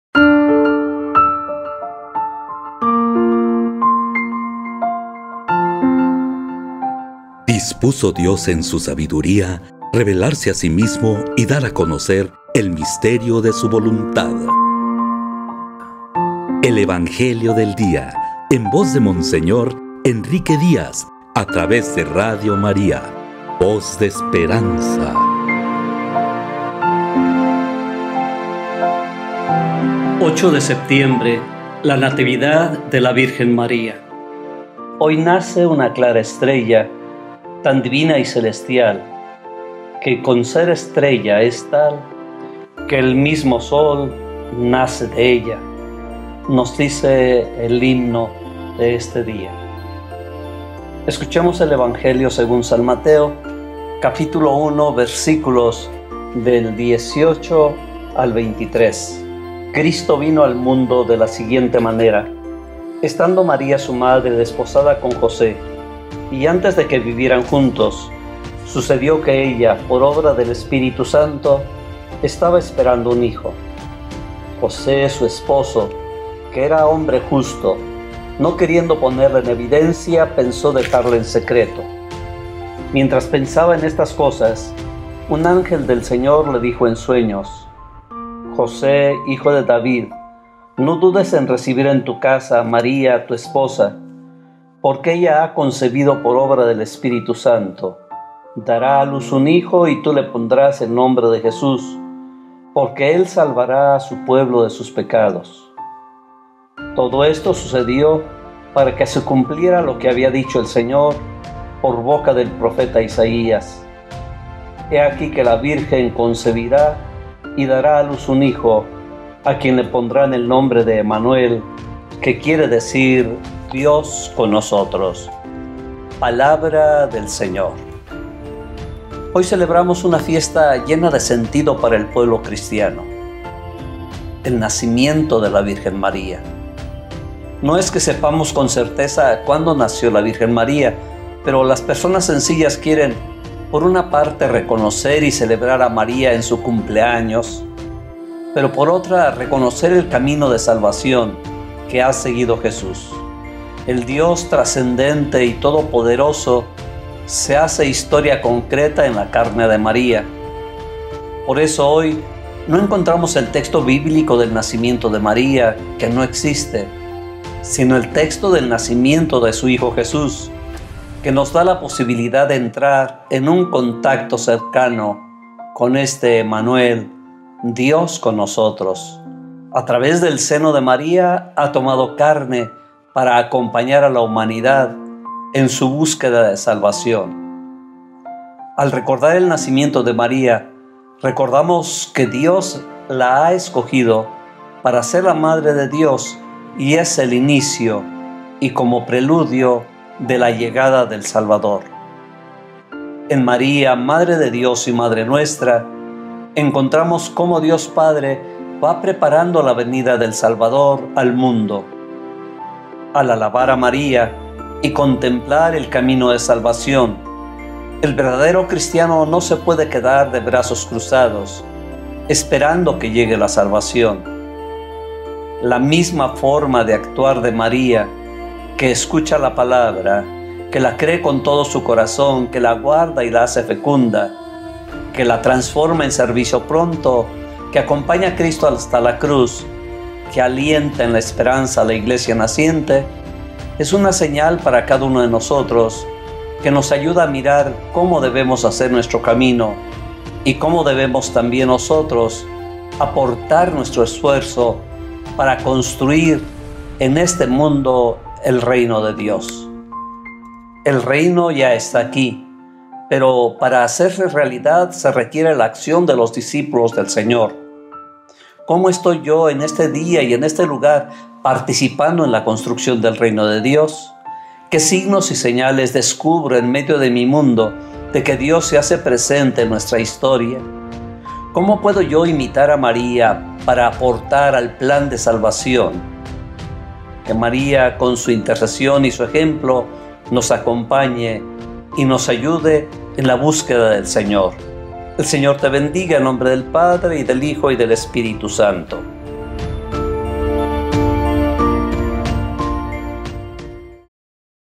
El Evangelio del Día | Mons. Enrique Díaz | 8 de Septiembre de 2020
Natividad de la Virgen María
San Mateo 1, 18-23: “La virgen concebirá y dará a luz un hijo”